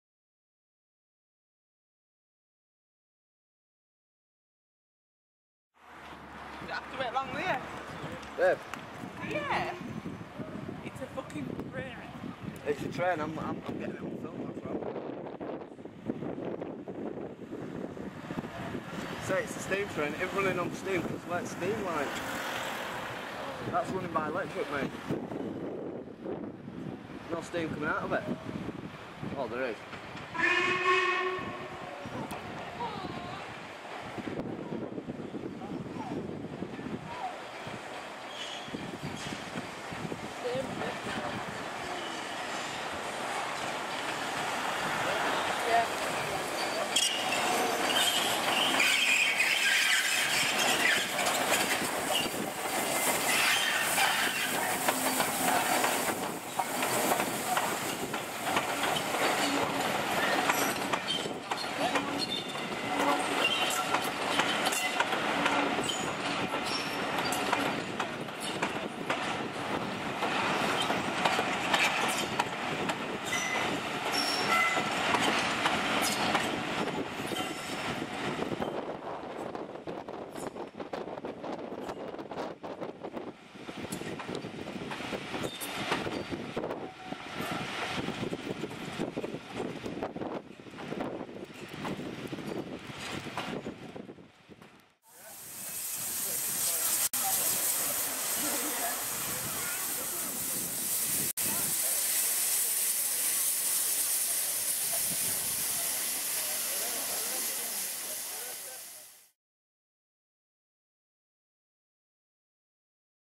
Scarborough Spa Express 46115 Scots Guardsman 22/07/2010
46115 Scots Guardsman entering Scarborough Station.
At the begining of this video you will here a couple talking a load of crap including the F word,i coaught up wit them about 10 mins after i took the video and gave them a few choice F words of my own LOL